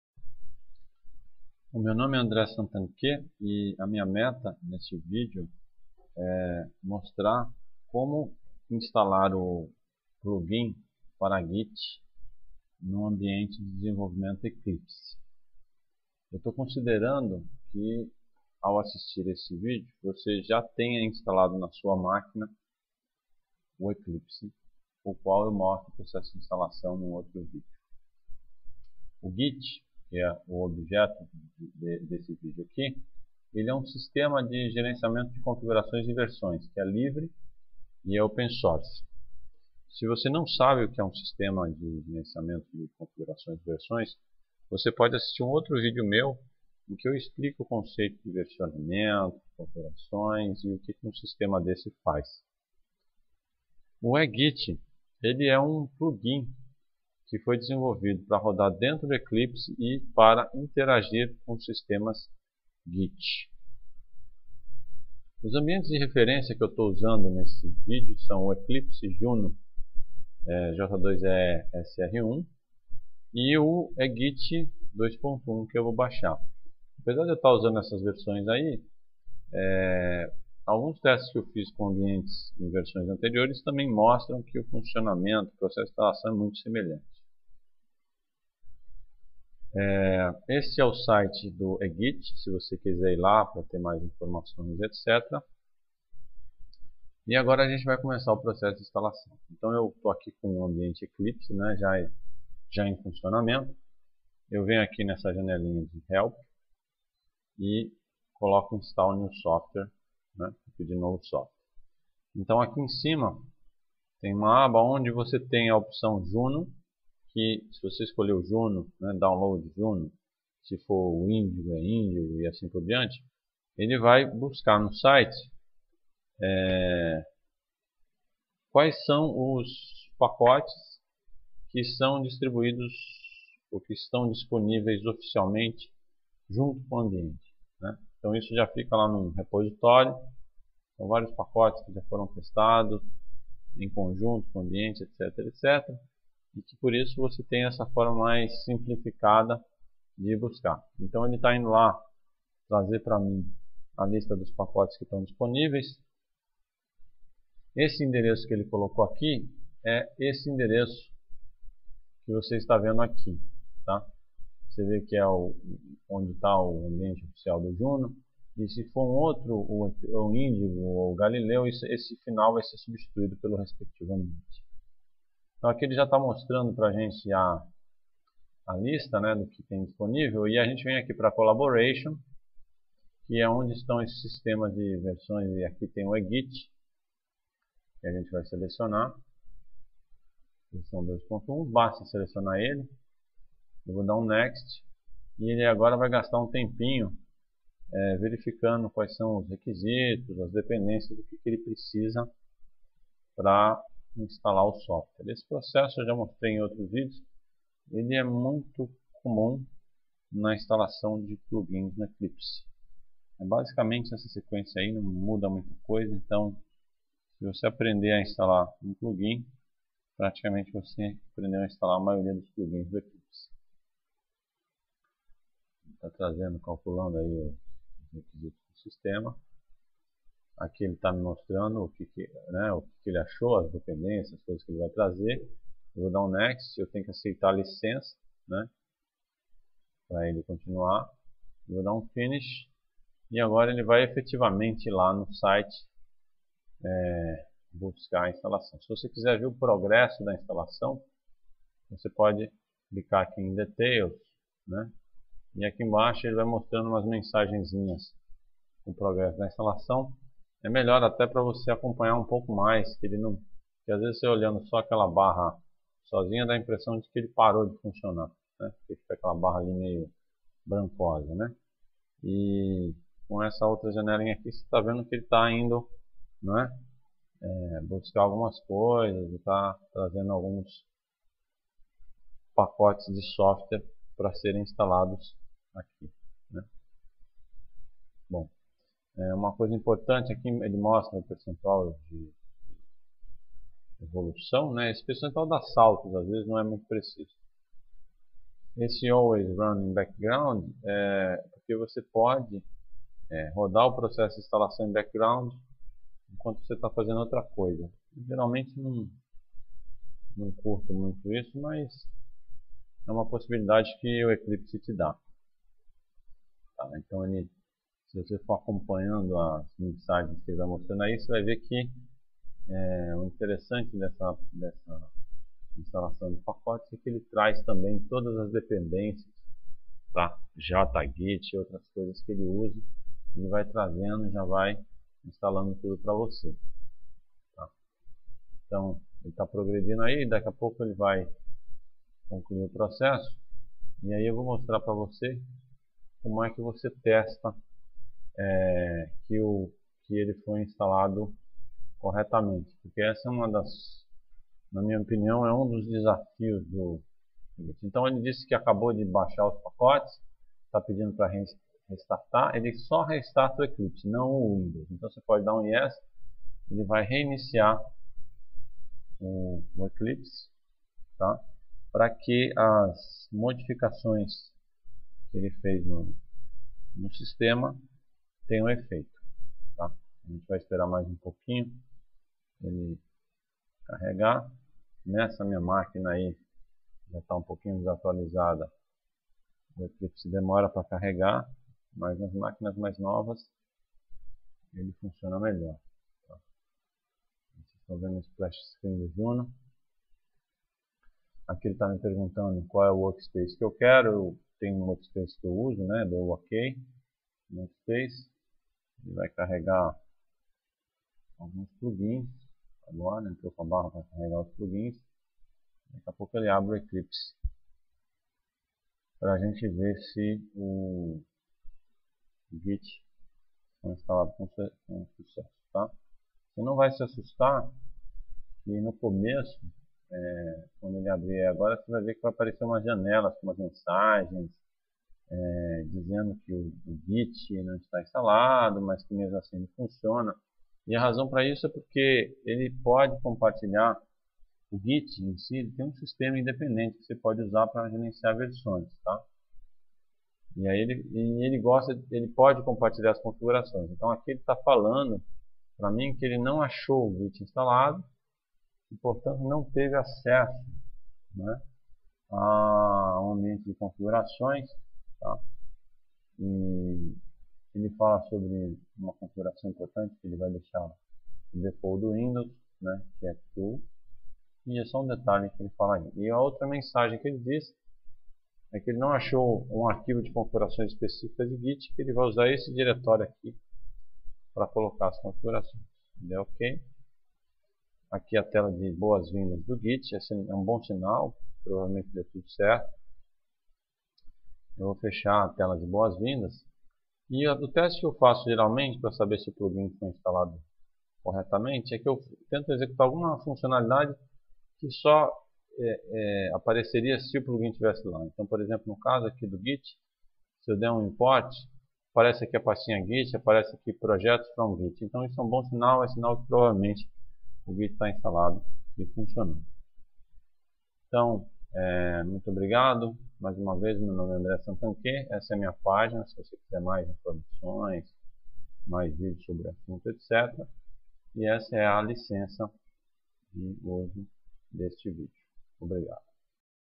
Instalando o plugin para Git no Eclipse
Detalha a instalação do plugin EGit no ambiente Eclipse Juno SR1. O EGit permite que o Eclipse interaja com sistemas de versionamento Git.